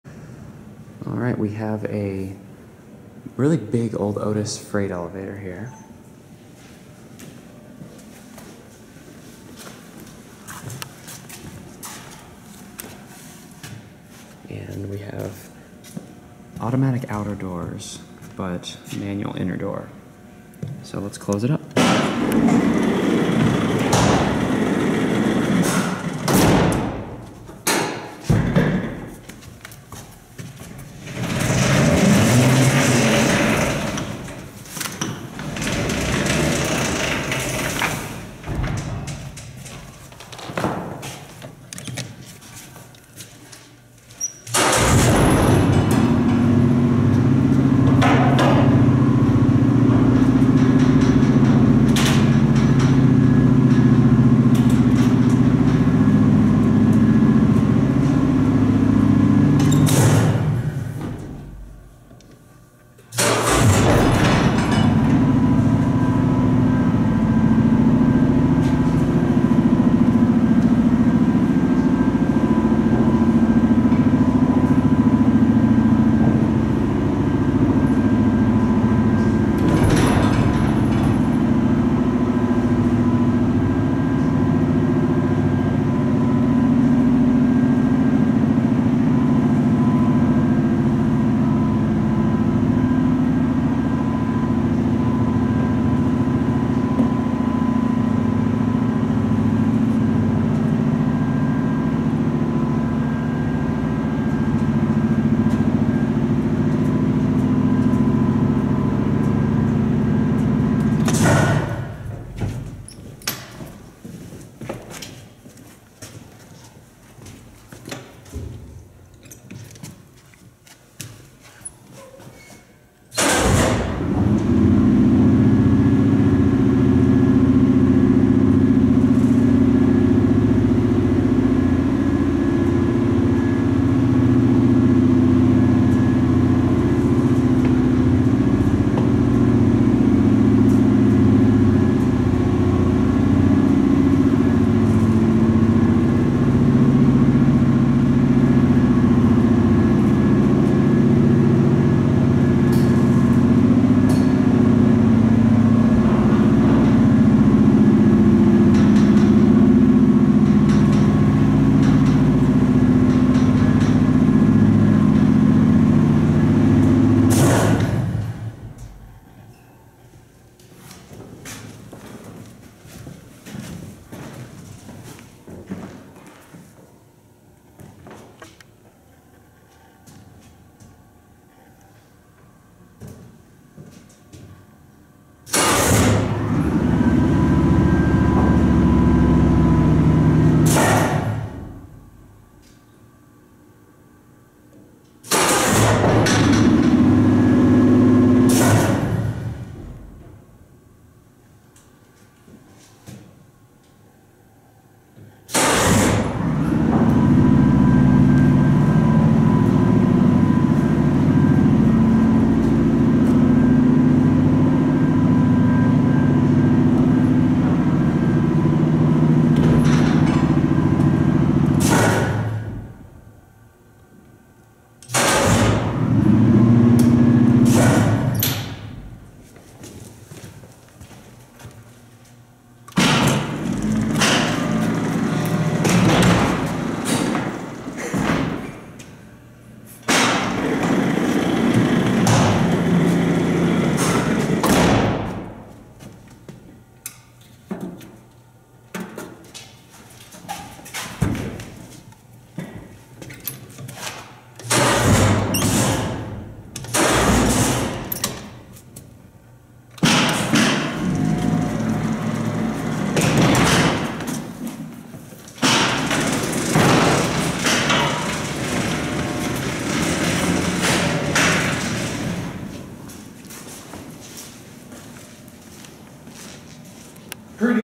Vintage Manual Otis Freight Traction Elevator #3
(1-5-21) This is the third and final Otis freighter in this complex. This one, like the others, is really awesome except it doesn't have as epic of a motor.